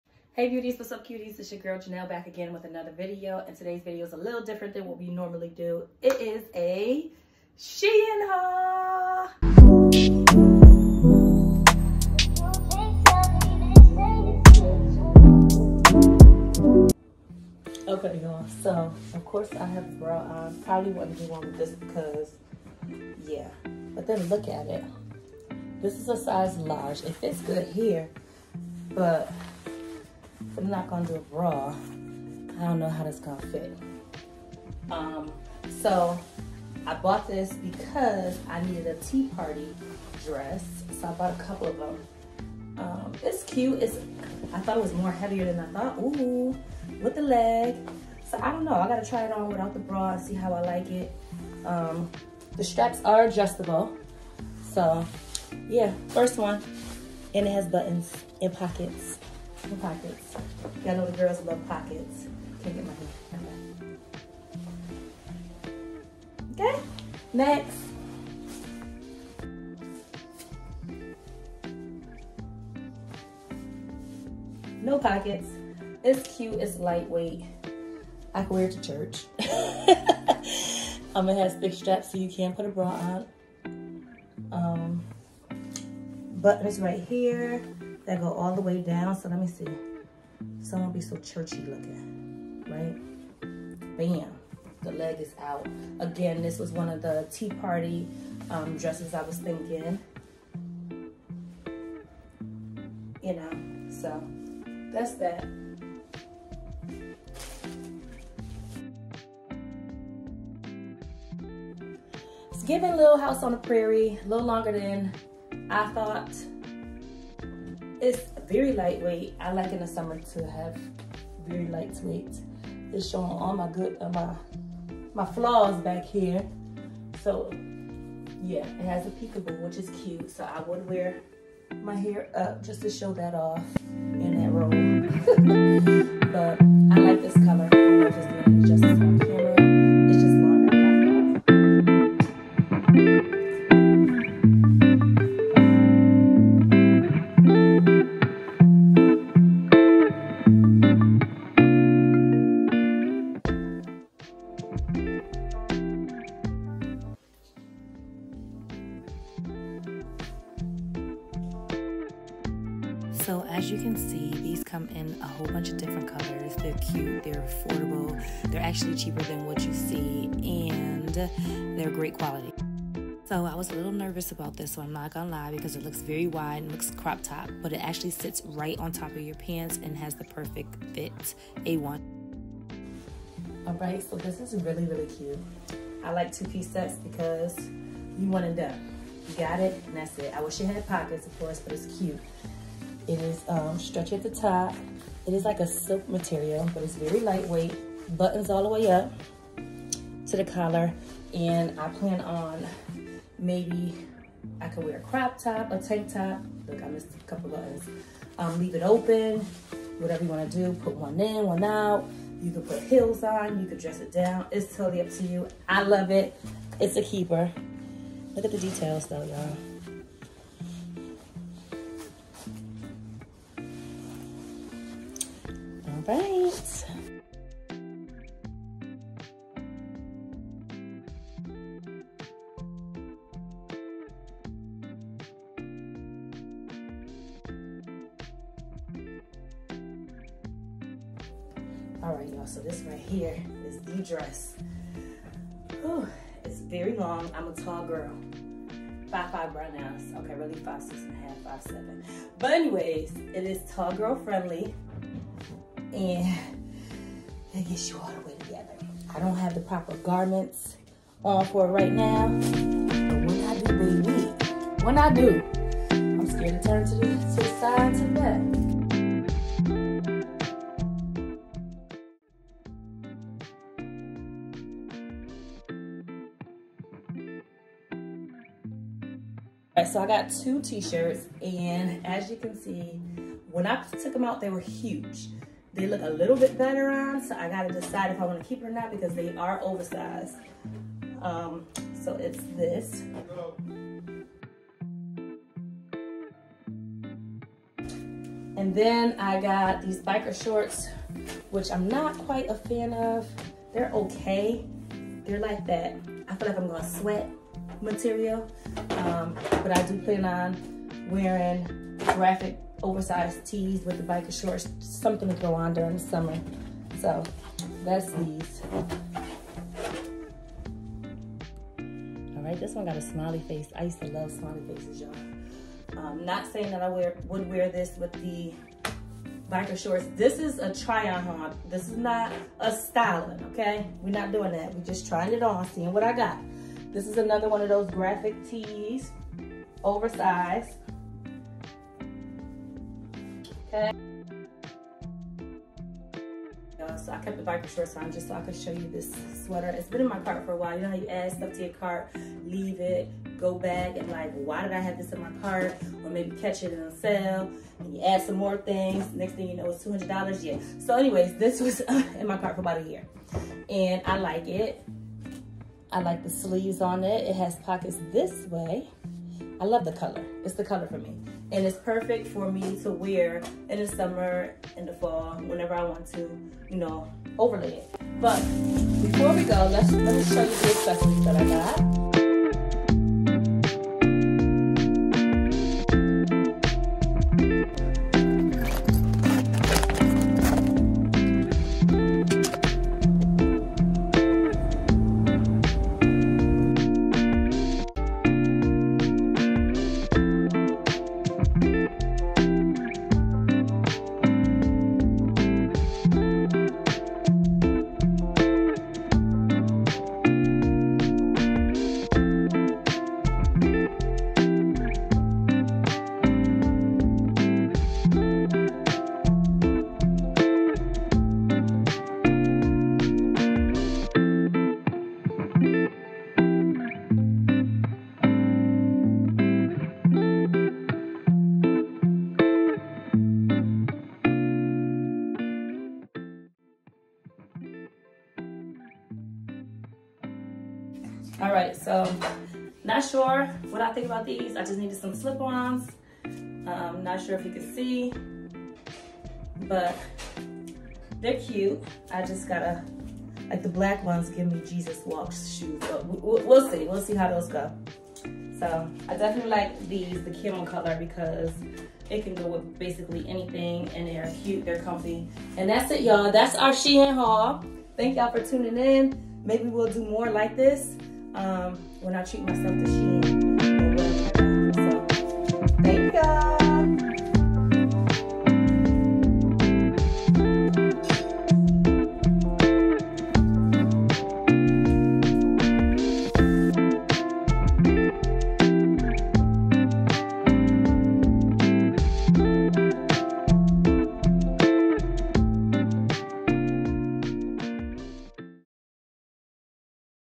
SHEIN SUMMER TRY ON HAUL
Try on Haul for SHEIN. I am normally a size M or Large. 5'7 with curves. Stay until the end to see sizes and prices. The black Fanny Pack you see is available